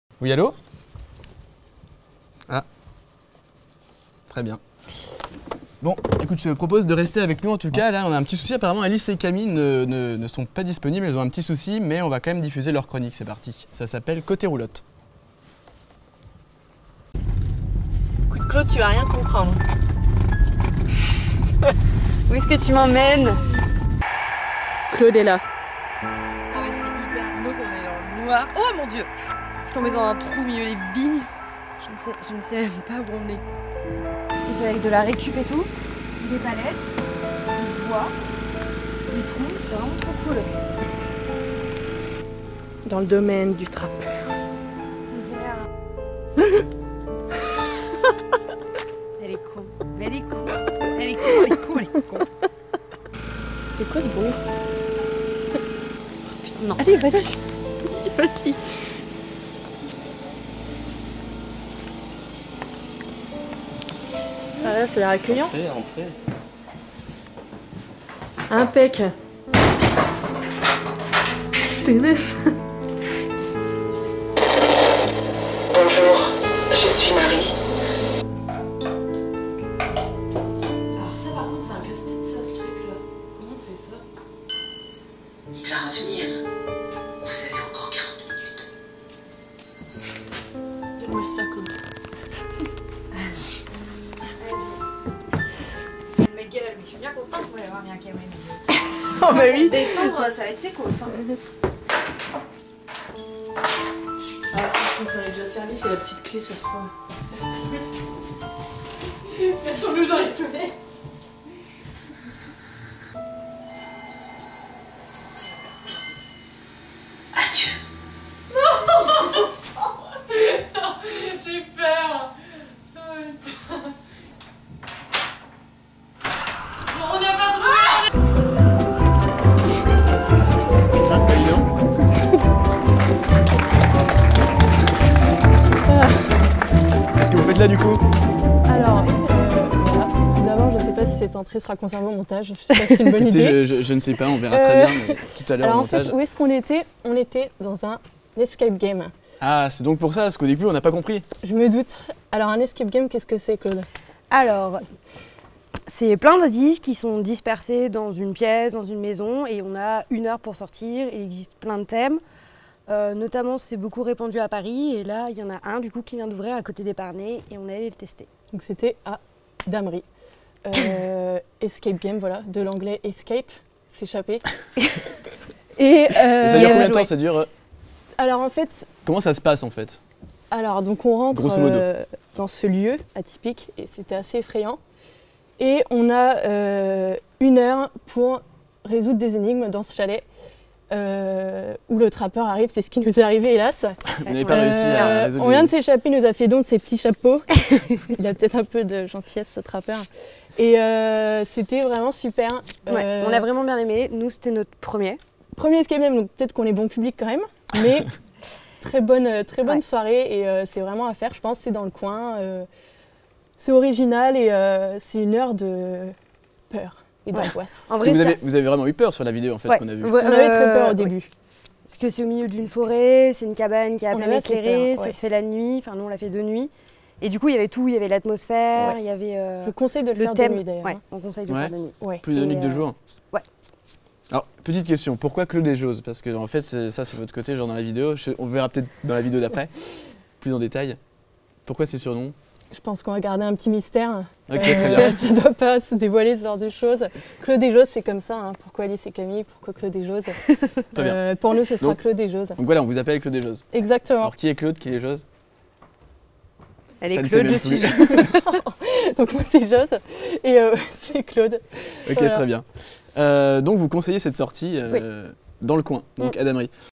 Côté Roulotte - Escape Game - La Grappe #1
Claude et Jos nous emmènent à la découverte d'un escape game à Damery !